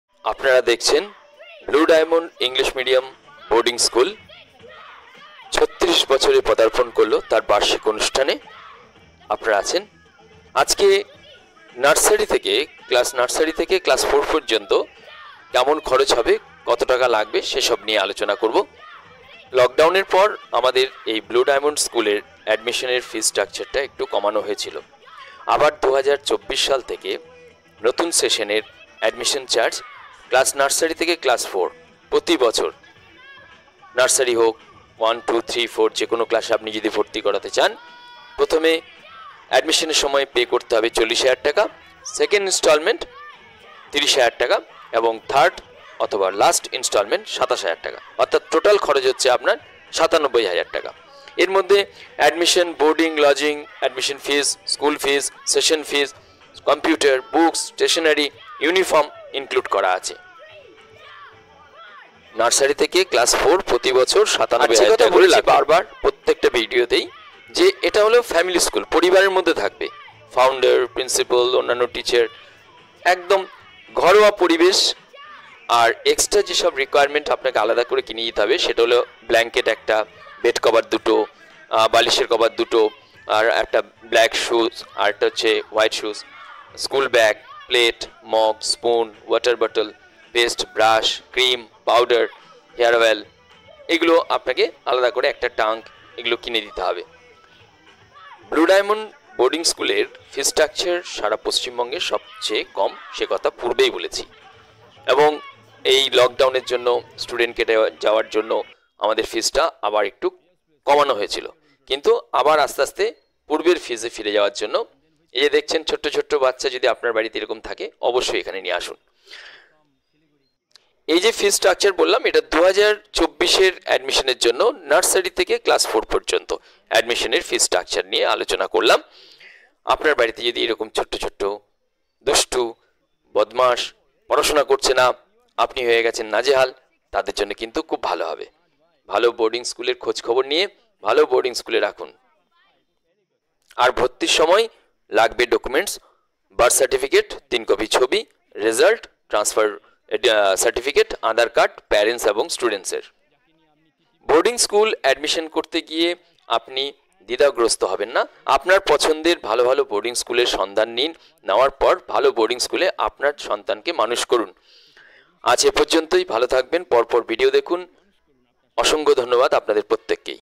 Best boarding school Fees structure
best boarding school fees structure in nursery to class 4 each class per year boarding lodging admission fee, school fee, session fee, computer class, books, stationeries and uniform single set includes all above Rs.97,000/-.
at the time of admission Rs.40,000/- rest amount instalment system.